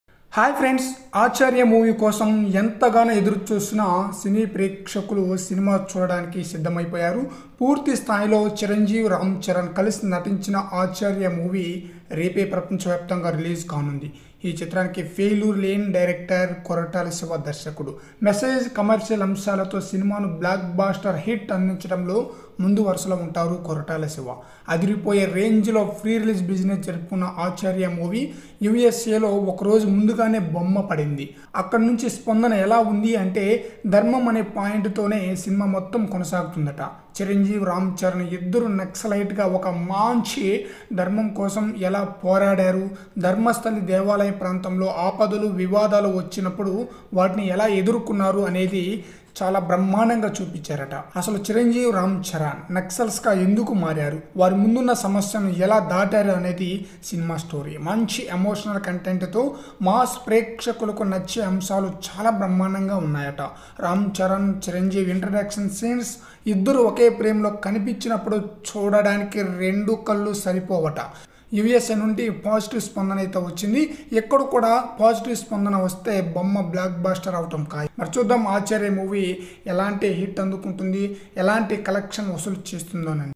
Acharya Movie USA Review | Acharya Public Talk
megastar chiranjeevi,ram charan kalisi natinchina movie acharya,koratala siva director,message and commercial amsalato cinemaku black blaster hit ivvtamulo koratala siva ditta,repe release kanunna acharya movie usa lo release ayindi,acharya movie usa review positive spandana vastundi.
acharya movie public talk kooda bagane undi.